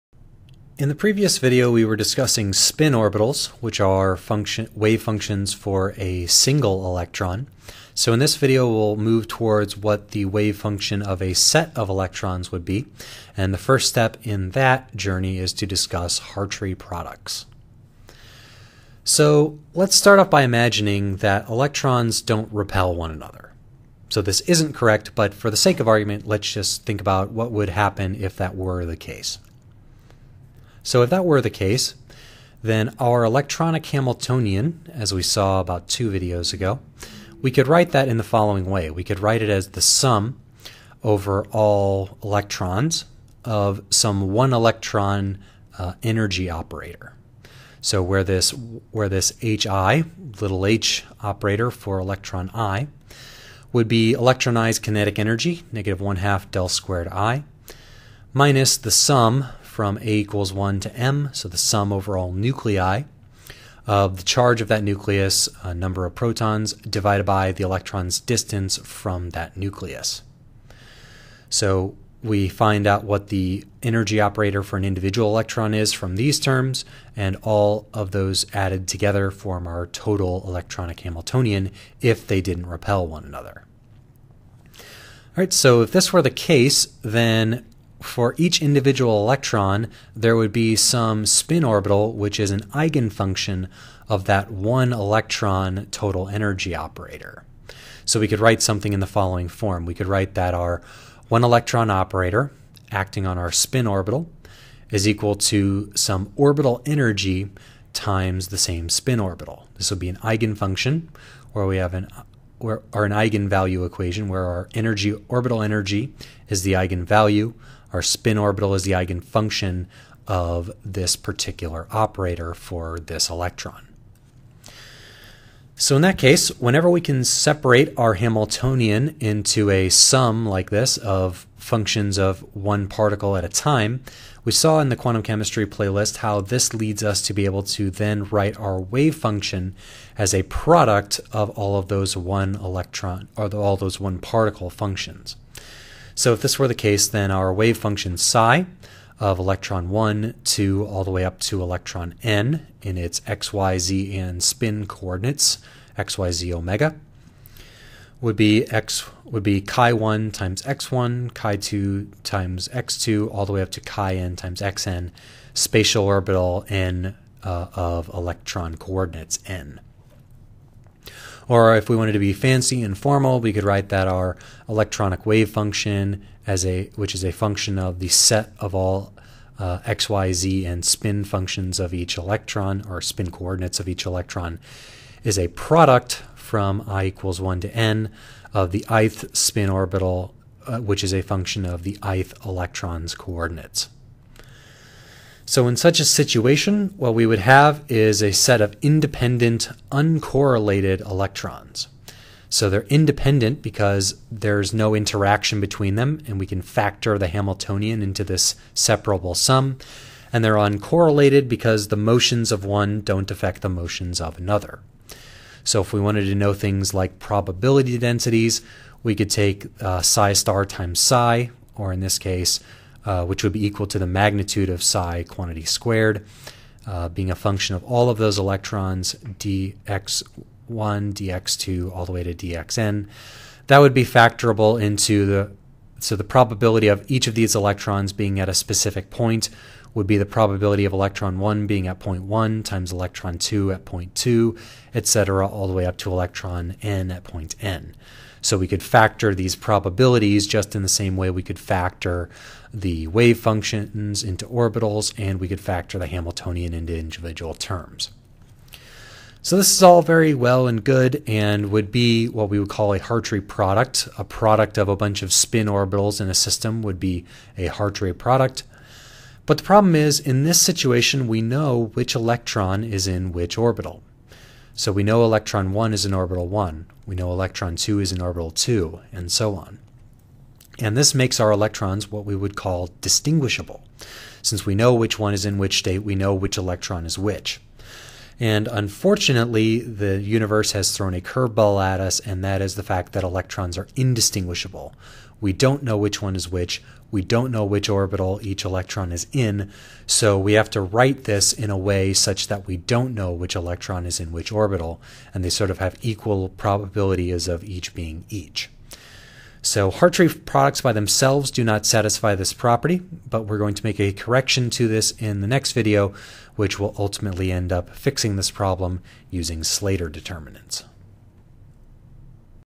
Computational Chemistry 4.5 - Hartree Product
Short lecture on Hartree-products of 1-electron spin orbitals.

A molecular system of N-electrons has a wavefunction which depends on all N spin coordinates (omega1 - omegaN) and 3N spatial coordinates [(x1, y1, z1) - (xN, yN, zN)]. The simplest way to approximate the form of such an N-electron wavefunction is as a Hartree product of N one-electron spin orbitals. In an approximate system where electrons are independent (i.e. they don't repel one another) this is an exact solution. In such an uncorrelated system the total probability density function is a product of the probability density function of each electron's spin orbital. The primary problem with Hartree products are that we know exactly which electron is in which spin-orbital, and thus the electrons are "distinguishable".

--- Video Links ---

--- Social Links ---

--- Equipment ---

Drawing Tablet: Wacom Intuos Pen and Touch Small

Drawing Program: Autodesk Sketchbook Express